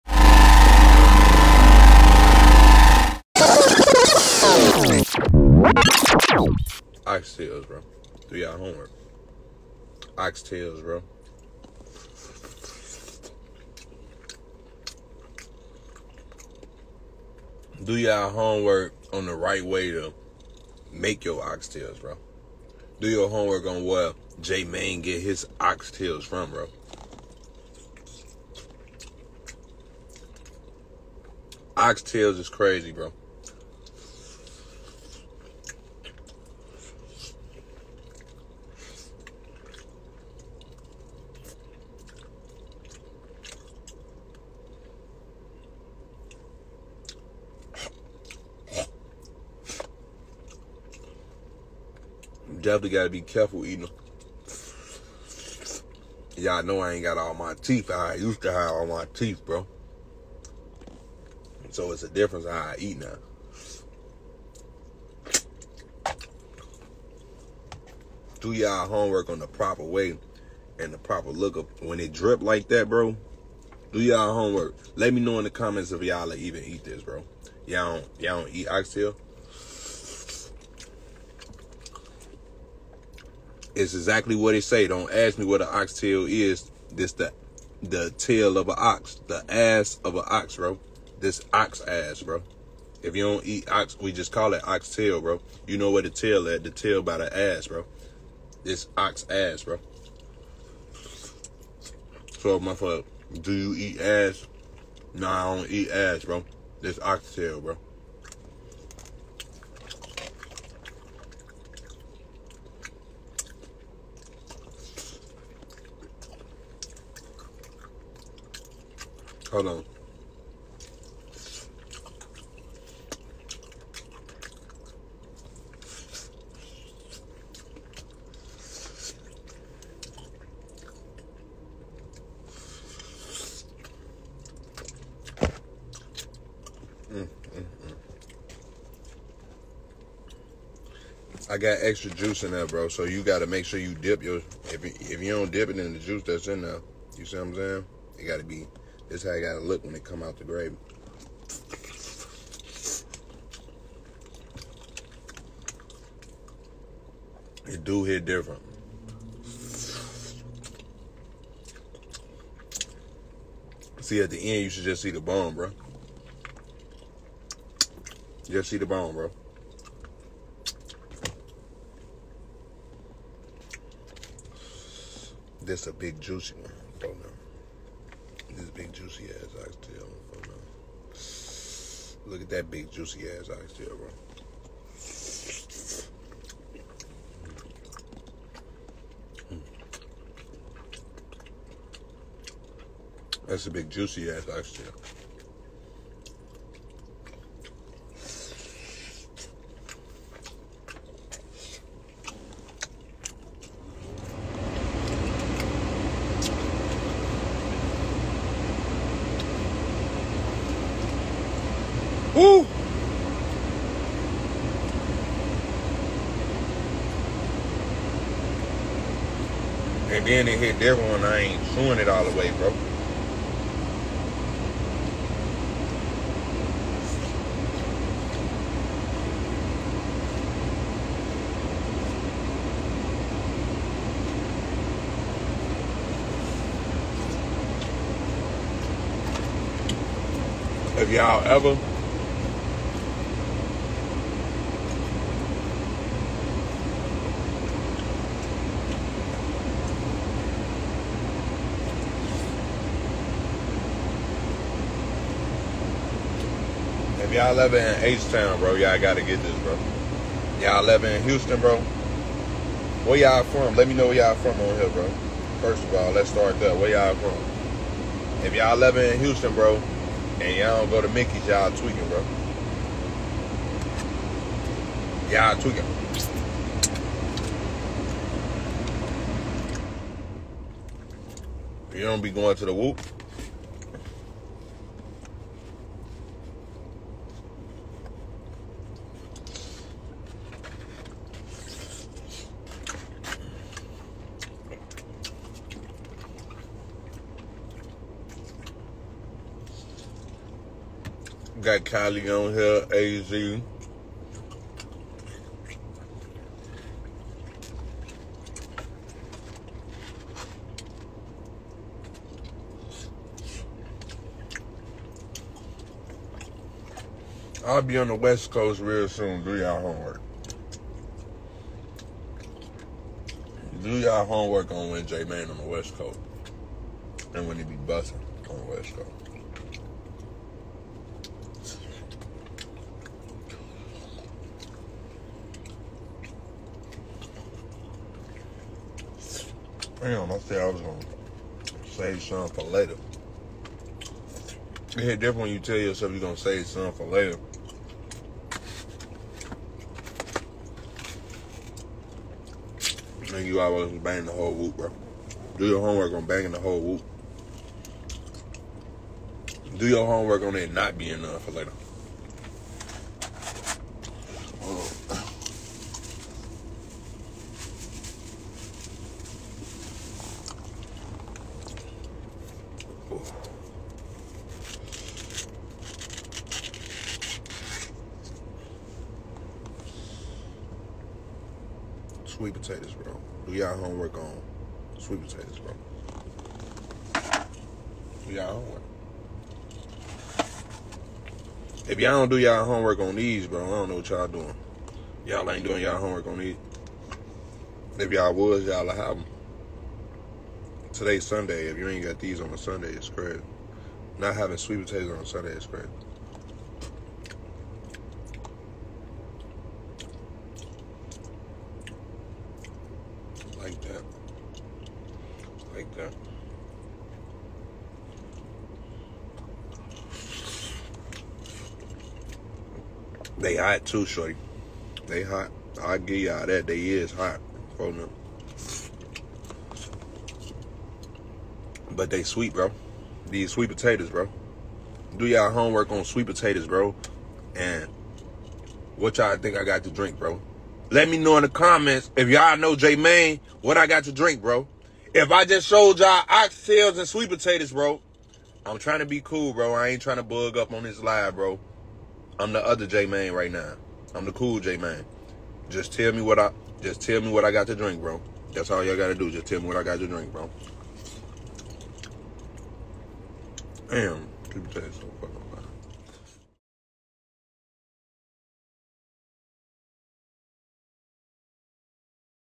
Fyb Jmane on ig live eating ox tails says it hit different
In this video I have fyb Jmane on ig eating oxtail talking about his experience.

TickTok- TayeRacks